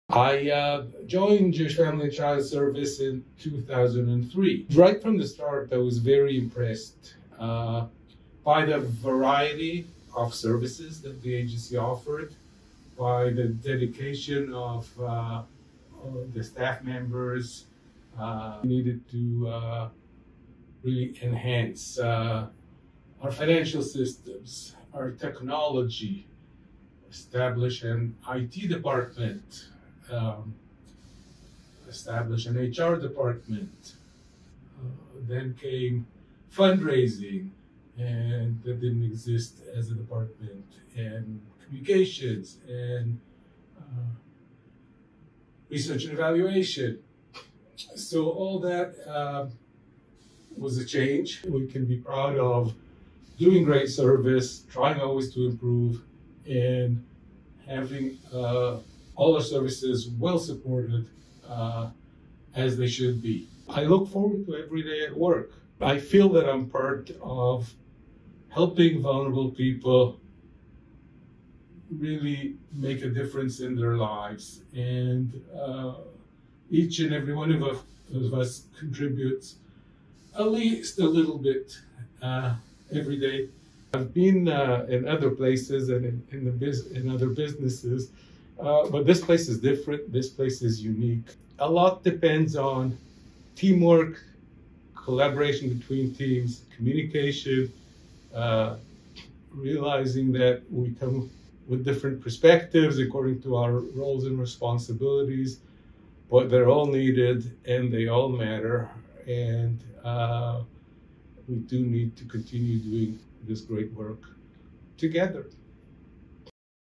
Zachy | A Day in the Life at JF&CS
JF&CS has been supporting the Toronto Jewish community for over 150 years. Throughout the course of our history, the agency has grown exponentially, extending our reach and growing our programs and services.

We want to share with you the many programs and services offered by JF&CS that support vulnerable members of the Jewish and broader communities.

Please watch the video to hear how adding new departments, improving our processes and remaining dedicated to our agency mission have allowed us to continue helping vulnerable and isolated community members.